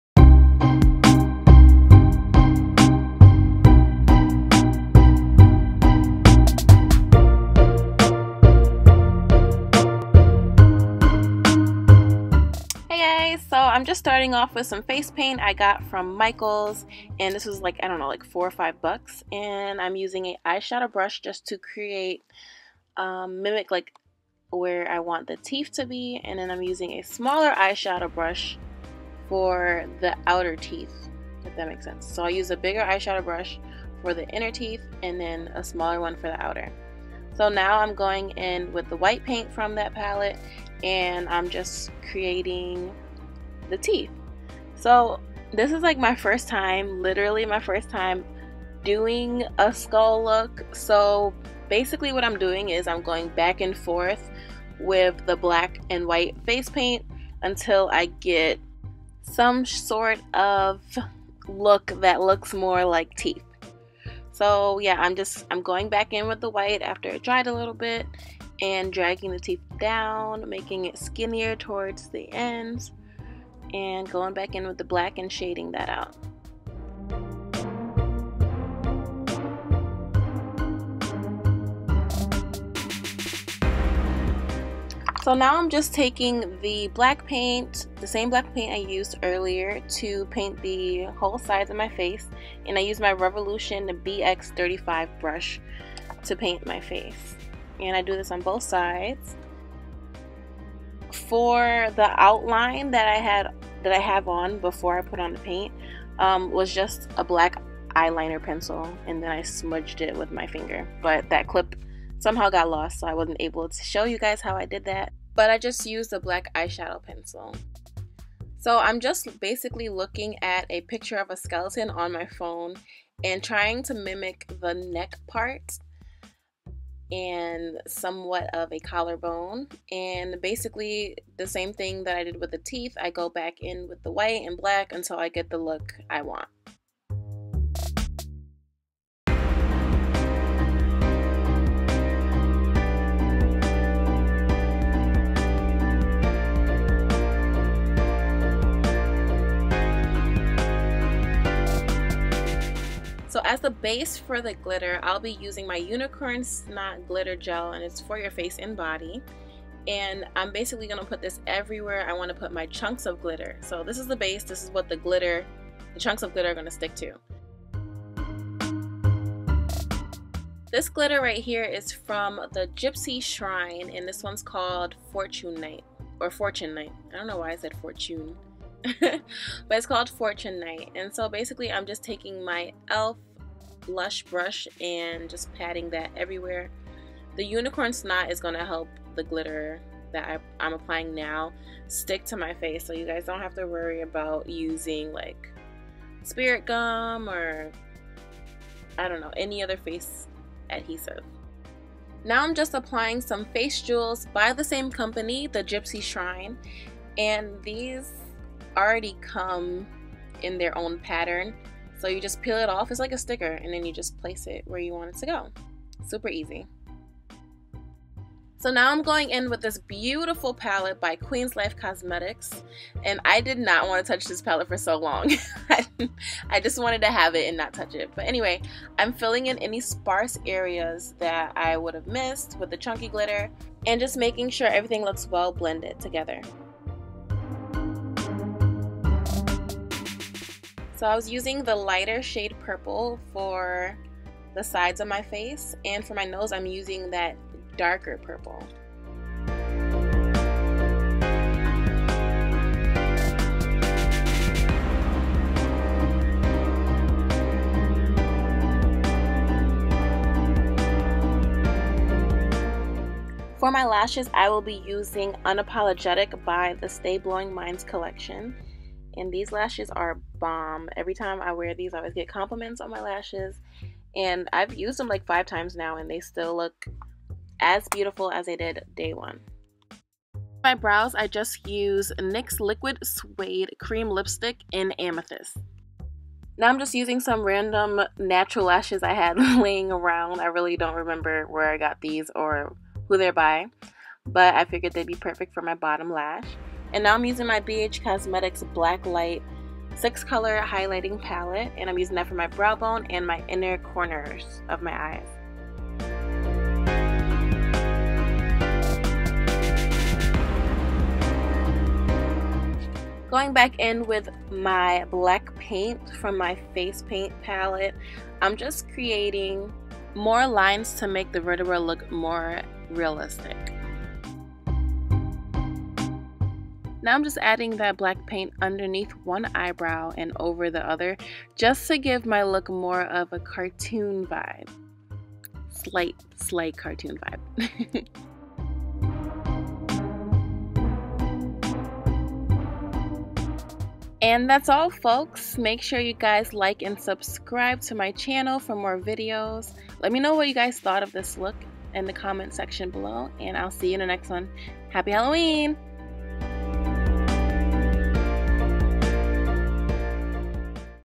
Glitter Skull Makeup | Halloween Tutorial | China Bell
This Purple Glitter Skill Look will SLAY any Halloween Event! Glow baby Glow!